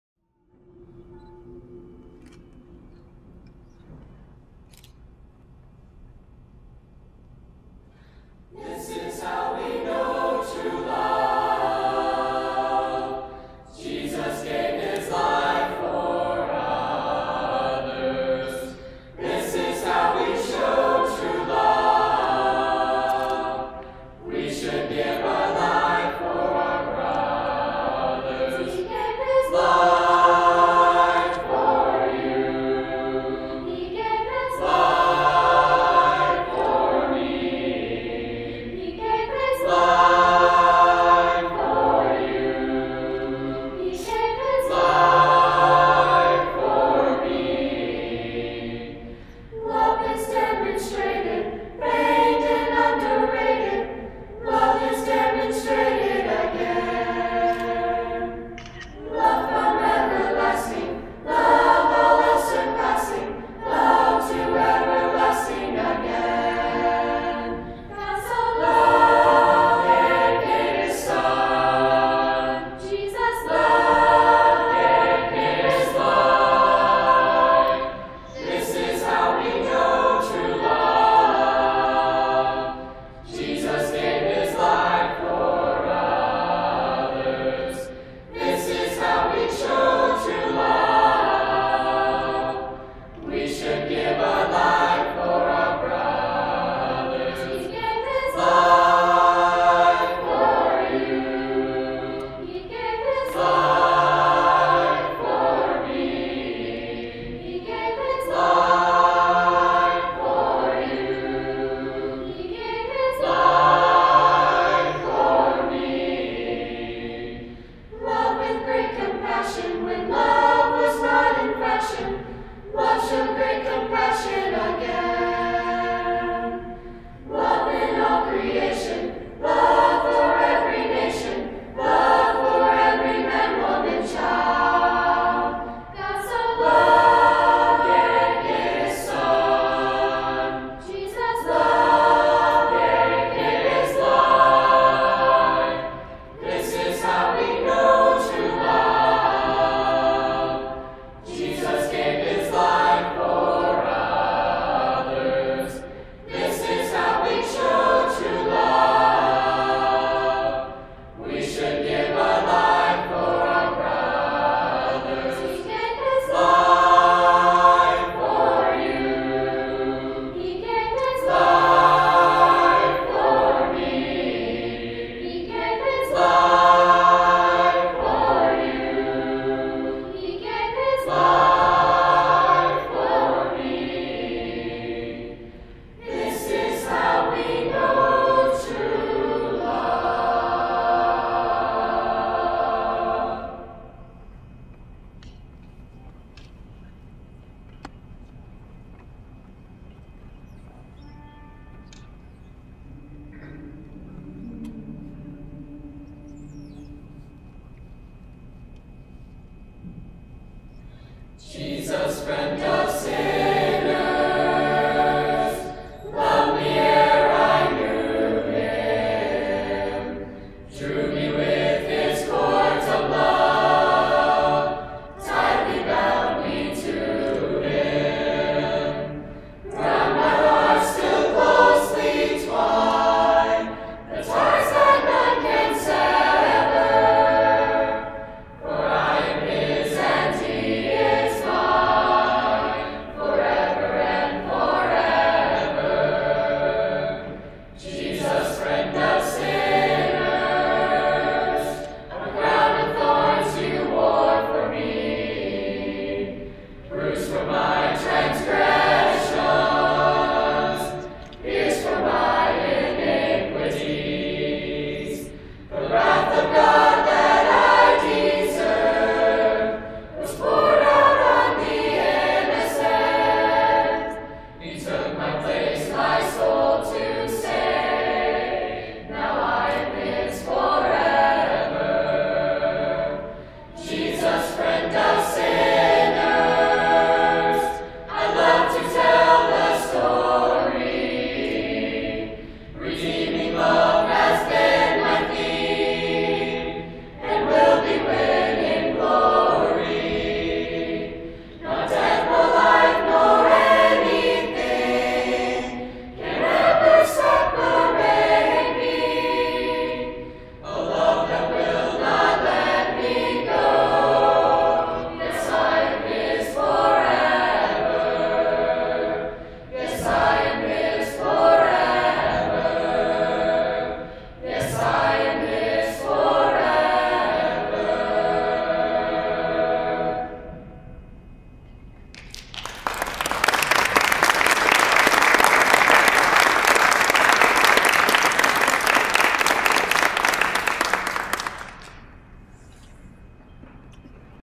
2015 LTC Chorus
This is the Church of Christ on McDermott Road's 7th - 12th grade Large Chorus.  The theme this year was taken from the book of Luke, 'Give It Up.'